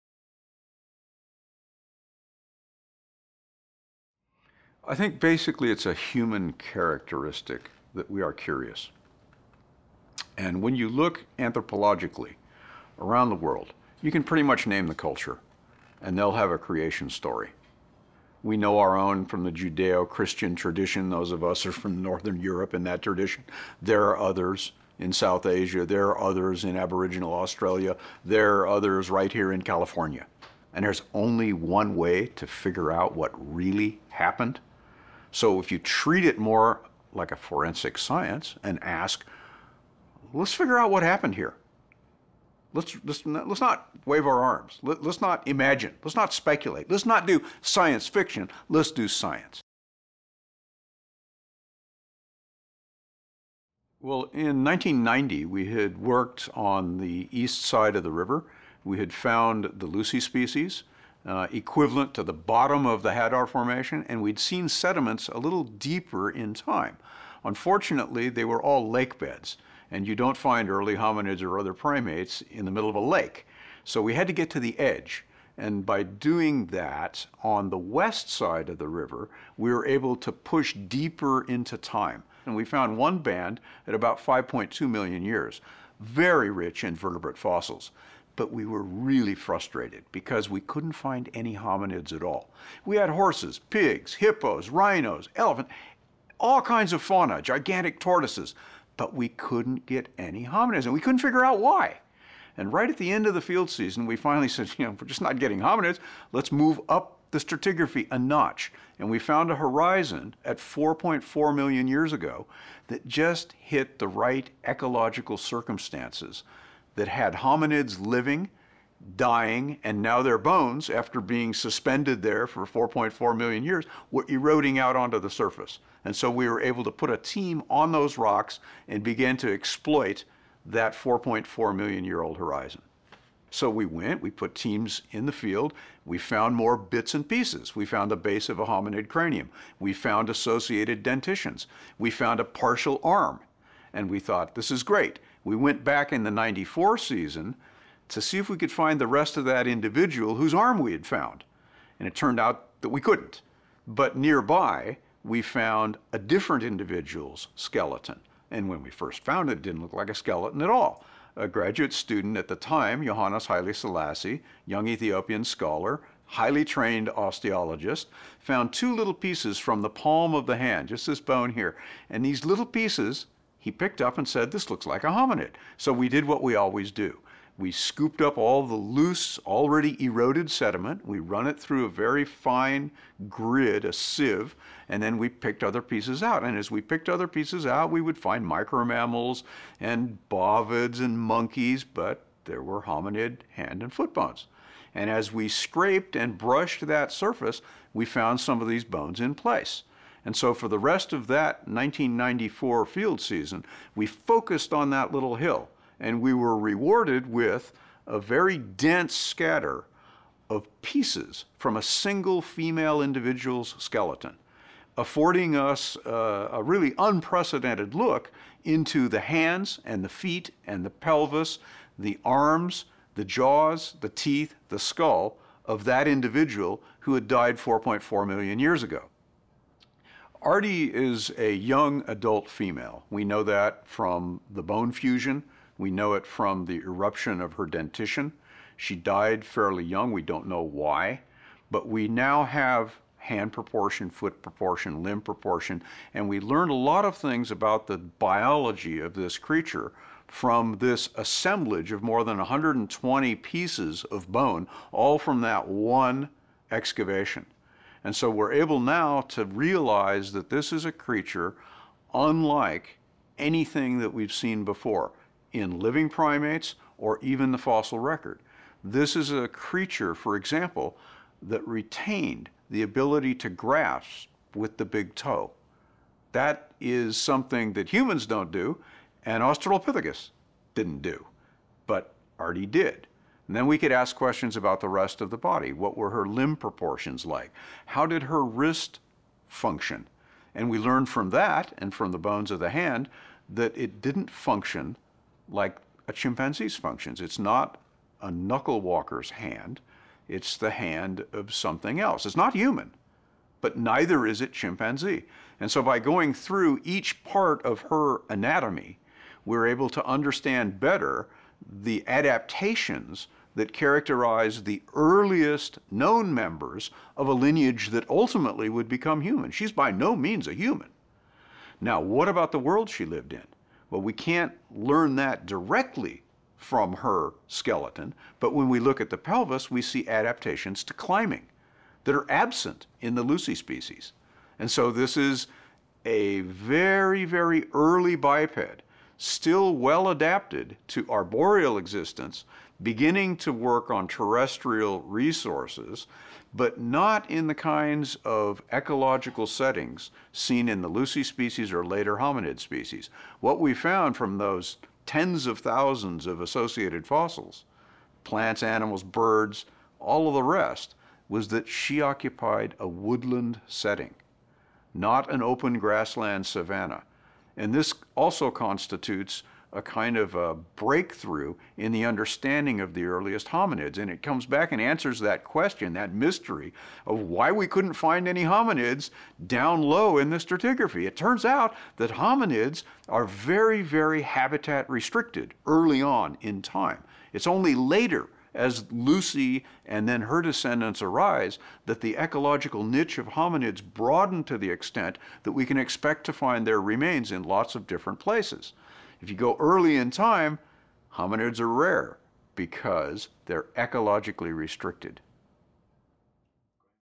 Tim White on the Intersection of Anthropology and Paleontology— HHMI BioInteractive Video
Paleoanthropologist Tim White discusses how his team unearthed the fossil of Ardipithecus ramidus, an early hominid that lived about 4.4 million years ago. In this classroom-ready science video, Dr. White discusses the fossil record and what it reveals about hominid evolution.

For more information about this discovery, visit the BioInteractive